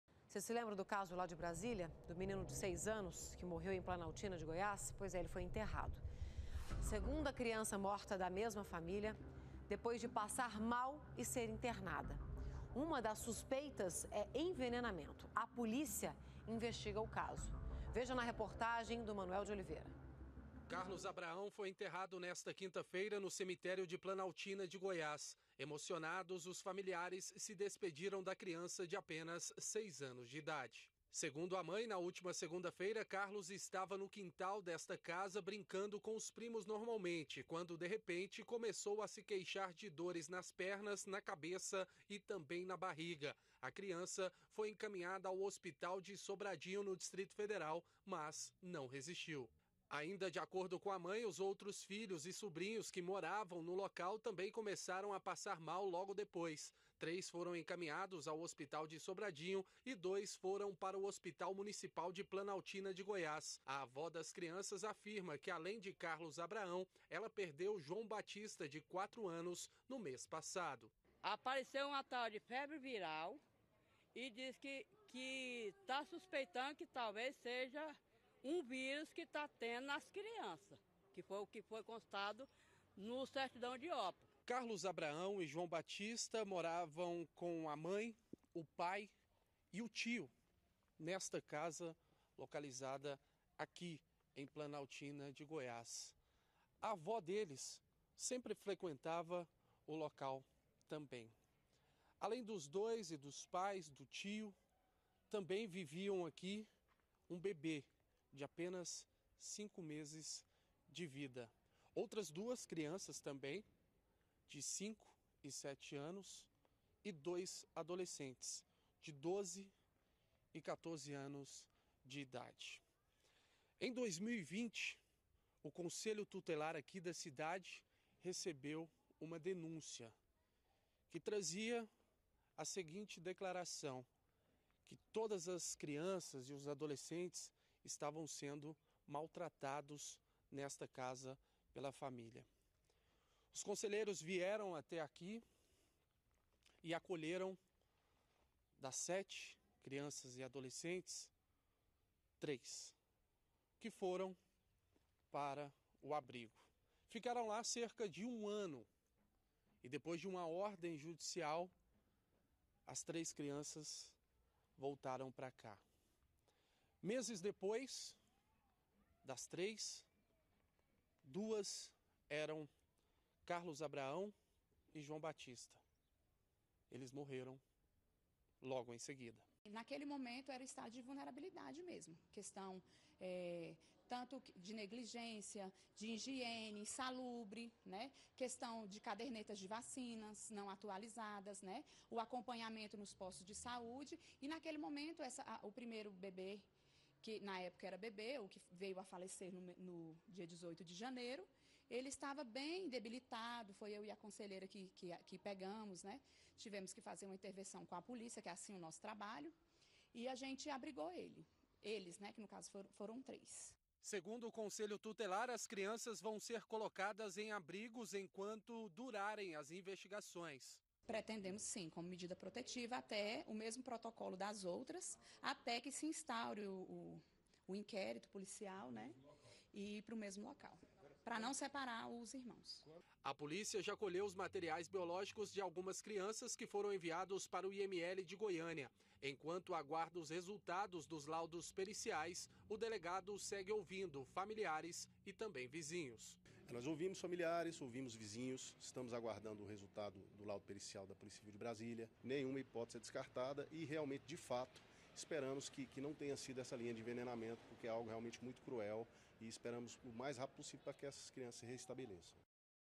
SUSPEITAS DE ENVENENAMENTO: MENINO DE SEIS ANOS É ENTERRADO EM PLANALTINA DE GOIÁS
SEGUNDA CRIANÇA DA MESMA FAMÍLIA MORTA, DEPOIS DE PASSAR MAL E SER INTERNADA. UMA DAS SUSPEITAS É ENVENENAMENTO. A POLÍCIA INVESTIGA O CASO.